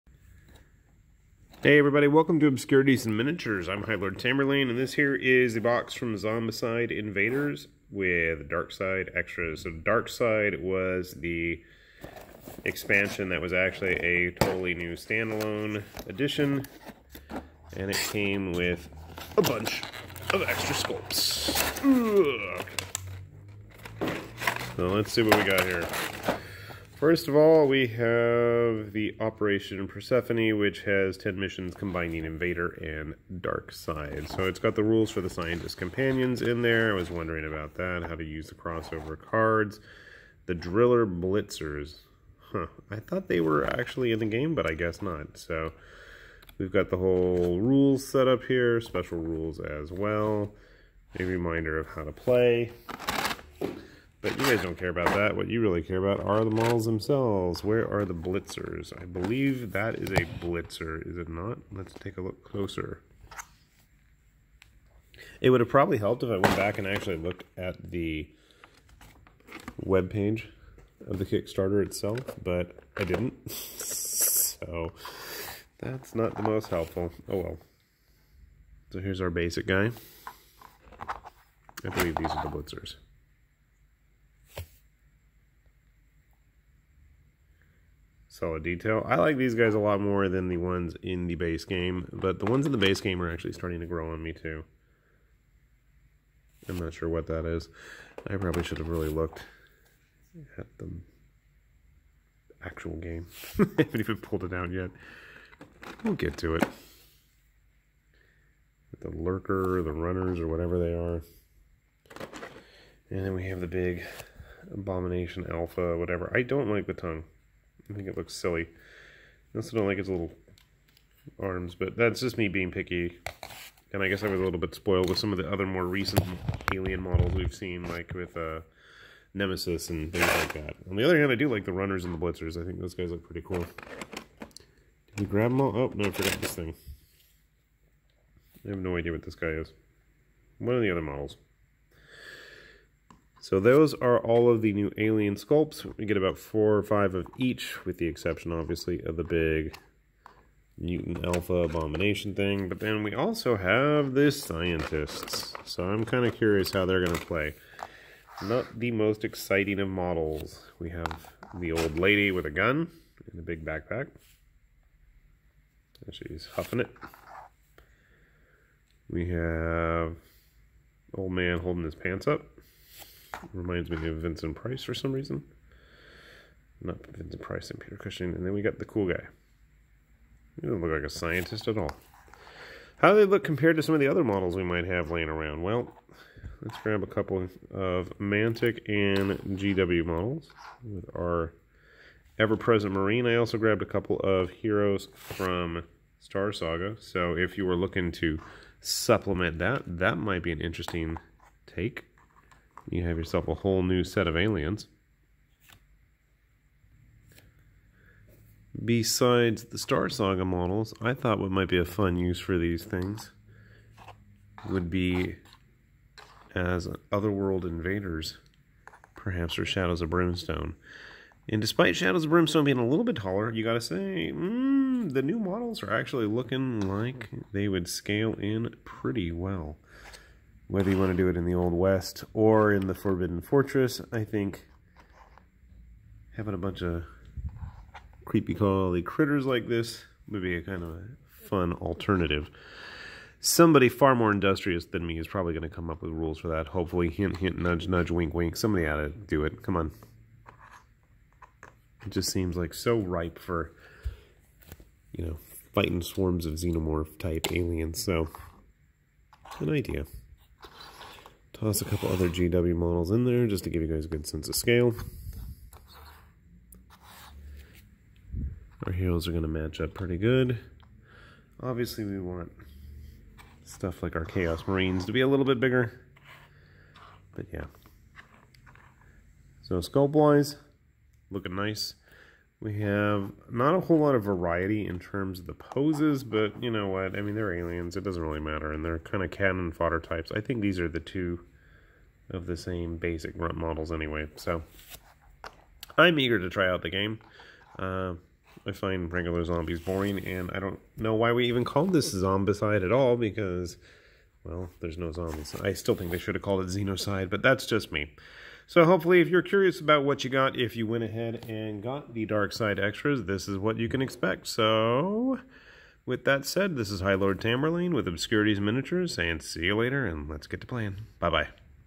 CMON: Zombicide Invader Dark Side Extras unboxed!
Let's take a quick look at the extras included in the Dark Side!
I have ulterior plans for these new Xeno Zombicide models...
Let's see how they compare to some of the other models in our collection while we're at it!